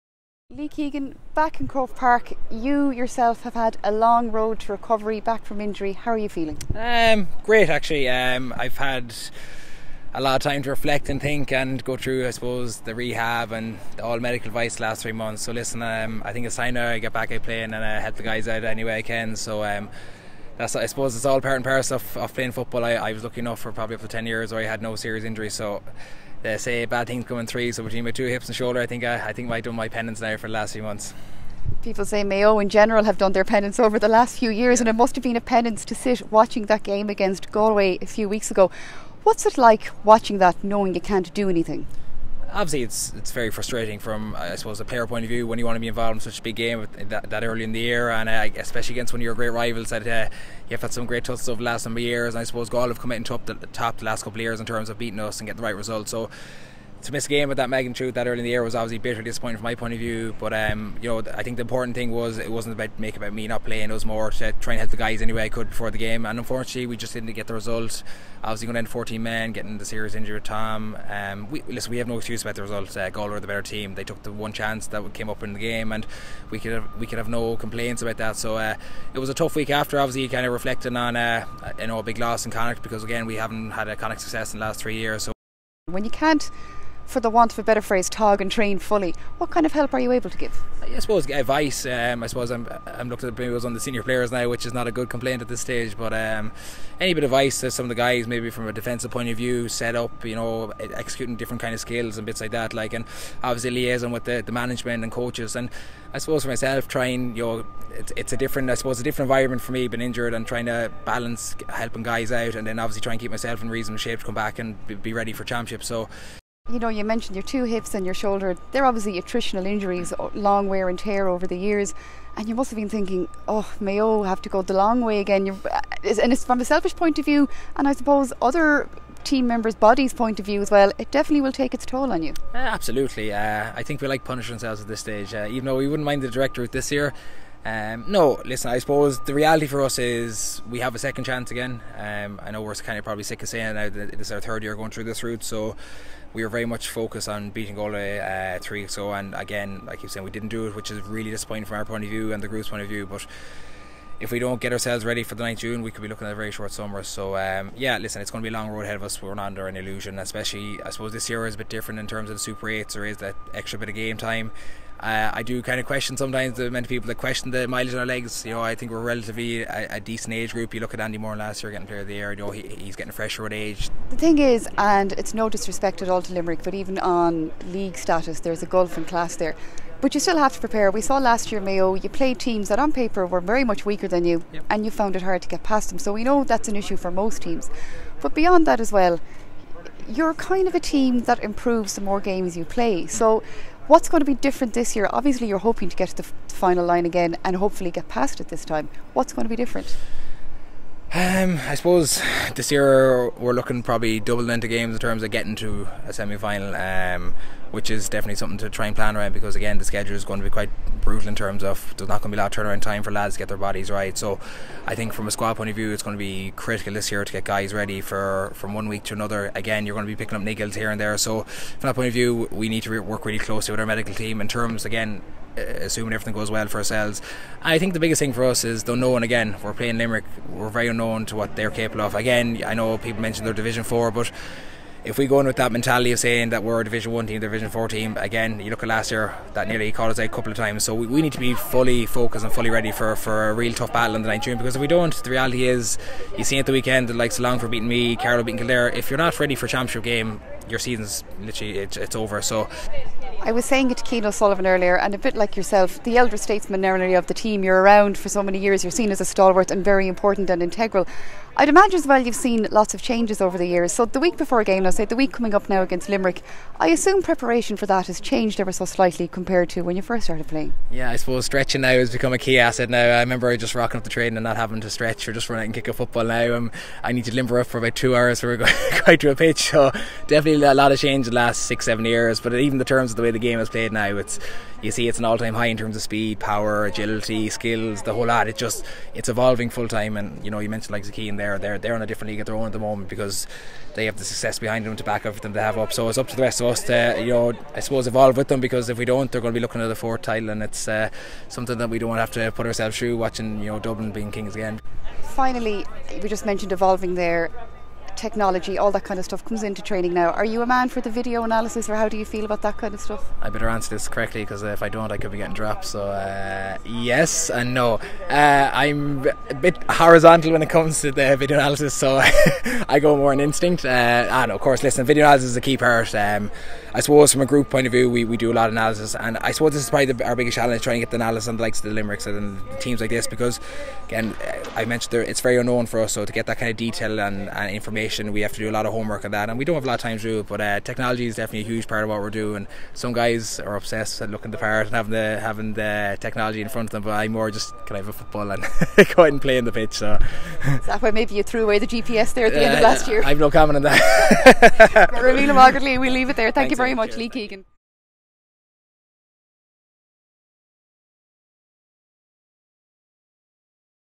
Lee Keegan: Injury update, chasing Dublin, brutal schedule, preparation, depth
Mayo's Lee Keegan spoke OTB about the season ahead